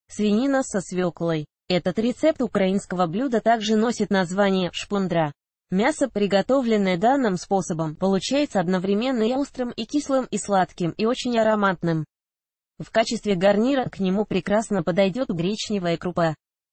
Свинина со свеклой
Этот рецепт украинского блюда также носит название «шпундра».